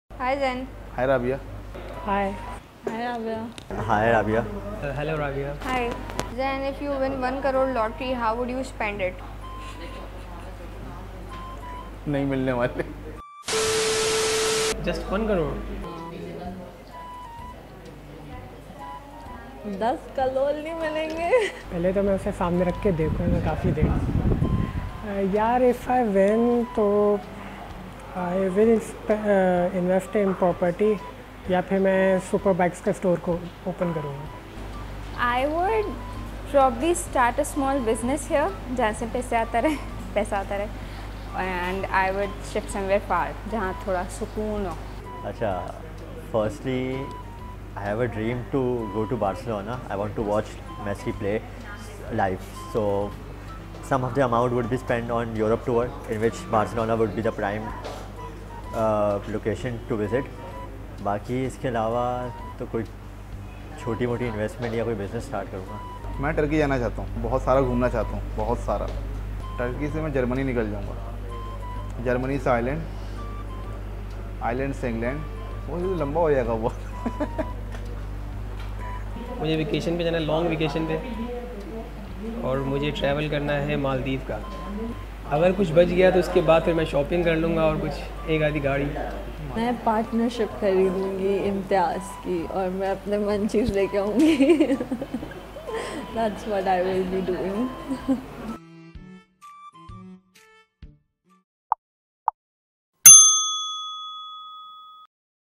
If you win 1 crore lottery how would you spend it? | TDF Tales
If you are given 1 crore, what is the first thing that you’ll do?

We asked the same from our team at TDF. Watch our space to know their wish list!